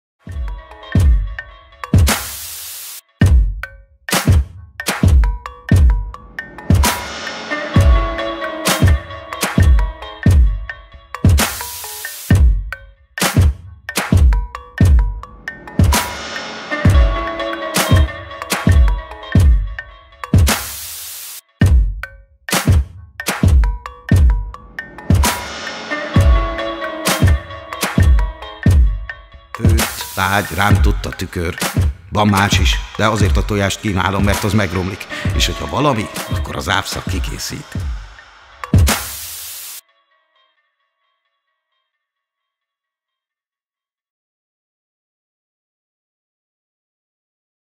HOLNAPELŐTT - trailer
Féltél már pincében? Akkor most!
Játsszák: Gémes Antos, Kovács Panka és Csiby Gergely
Rendező: Horváth Illés
Írók: Horváth János Antal és Horváth Illés
Videó: Koblicska Örs, Rónai Domonkos
Apokaliptikus bújócska a Magyar Színházban